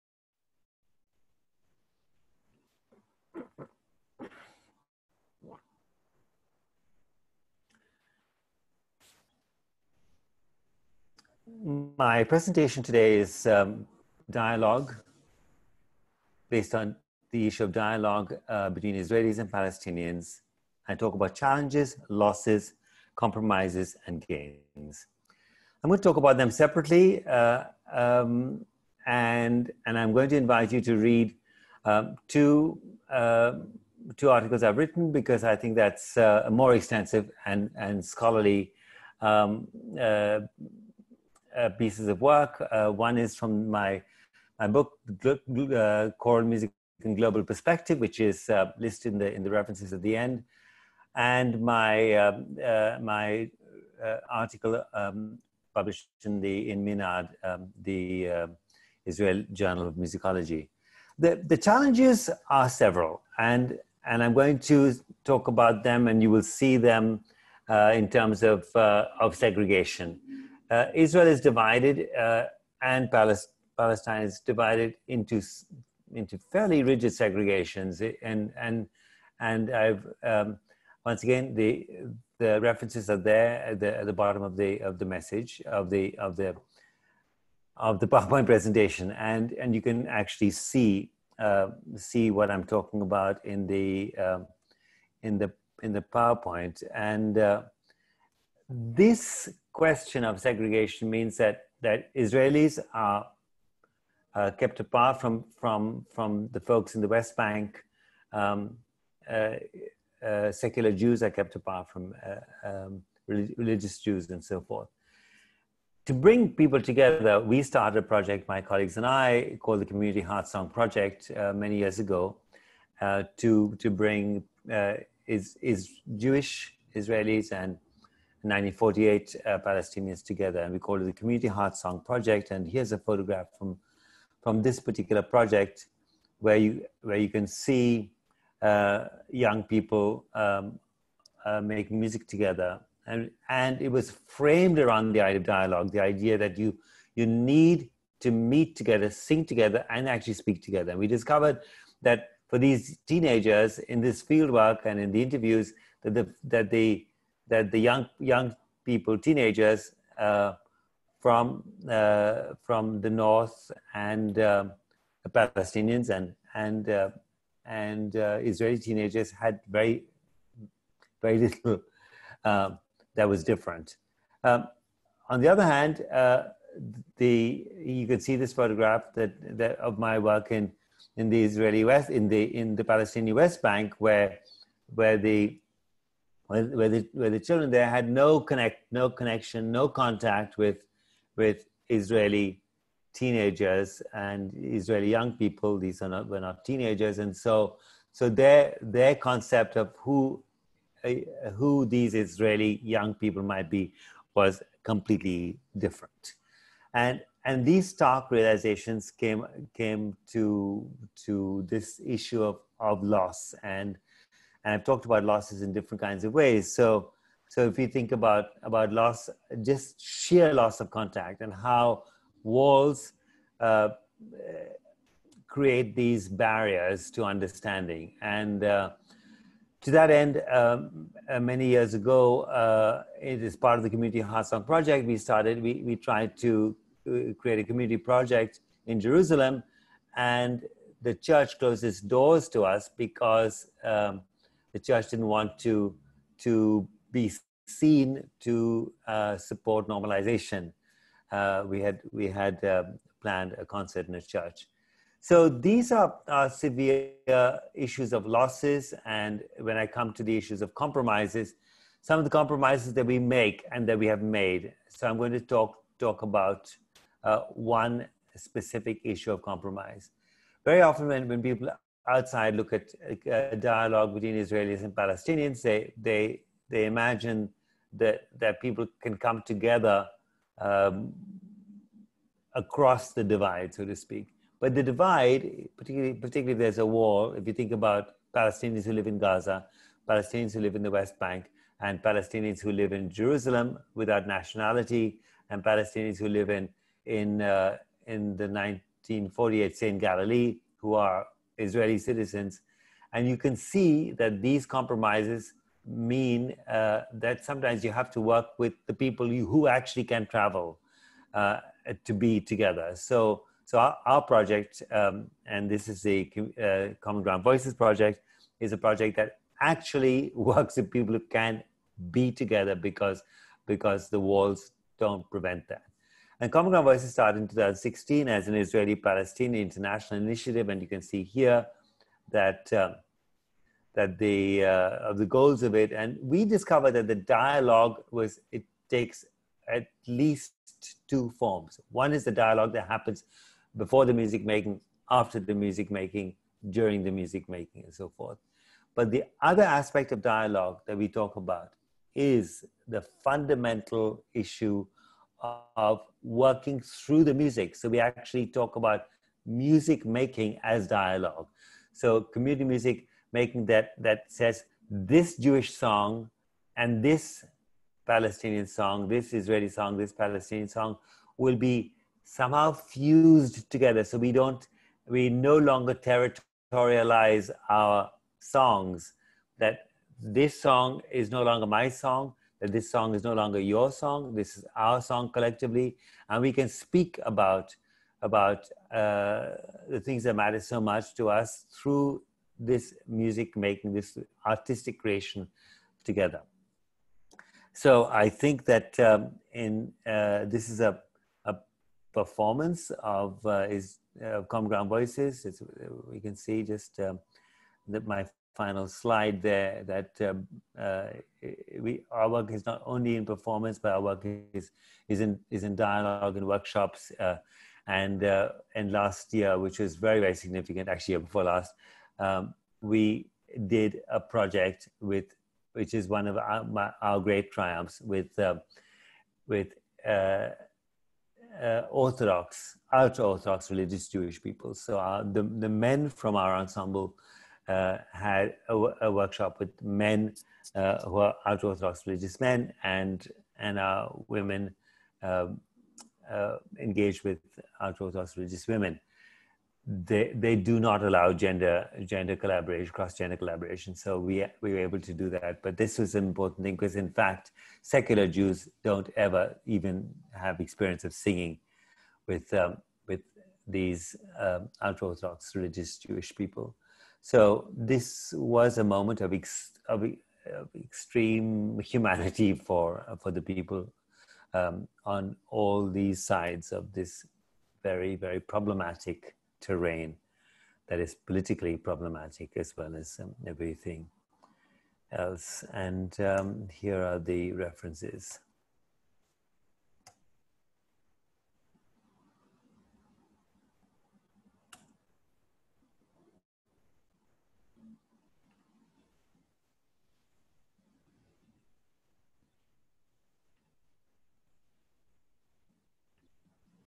Andre de Quadros - Dialogue through Musicking between Israelis and Palestinians
Dialogue through Musicking between Israelis and Palestinians: Challenges, Losses, Compromises, and Gains.
Since 2016, Common Ground Voices, a community ensemble based in Jerusalem has been a site for dialogue in the contested geography and politicized narratives between Israelis and Palestinians. While conventional wisdom would seem to dictate that intragroup dialogue is effective, between Israelis and Palestinians dialogue is regarded by even the most liberal, as potentially risky or useless at worst. Against this context, Common Ground Voices has established a musical and pedagogical process located in the Empowering Song approach derived from Boal’s Theatre of the Oppressed (de Quadros, 2018; de Quadros, 2019) that has resulted in varying outcomes.
This paper discusses the historic challenges encountered in assembling this ensemble and holding it together against formidable political and social resistance from within the group and from the community at large. Working through these challenges has resulted in some members of the group feeling that their priorities are inadequately served. While the Palestinians seek to have a discussion of Israeli occupation, some Israelis and Palestinians see such discussion as exposing fractures. The delicate balance of the ensemble’s emotional equilibrium is also examined through its engagement with Palestinians in the West Bank and with ultra-orthodox Haredim Jewish people in Jerusalem. These activities have given the ensemble a spectrum of engagement like no other. This paper discusses the ways in which the conversations are mediated through the music-making, and the way in which repertoire is selected, fused, and performed. These processes have necessitated a serious consideration of the asymmetrical power-dynamics within the group, and how the future may evolve in increasingly fraught political contexts.